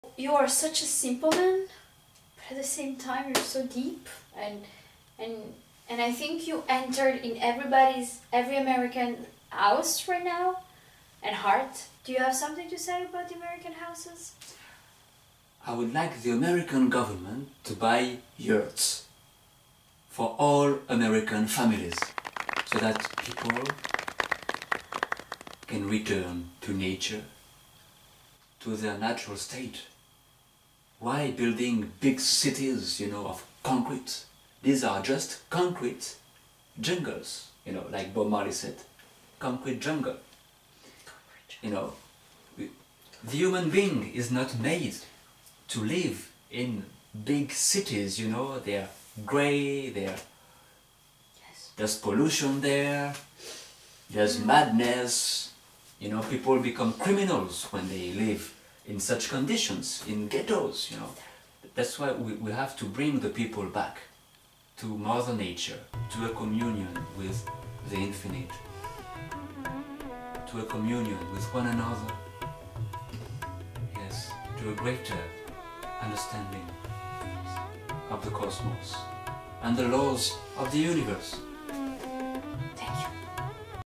Yurts for all americans with the Cosmic President !!!
His Holimess the Cosmic President promises yurts for all Americans !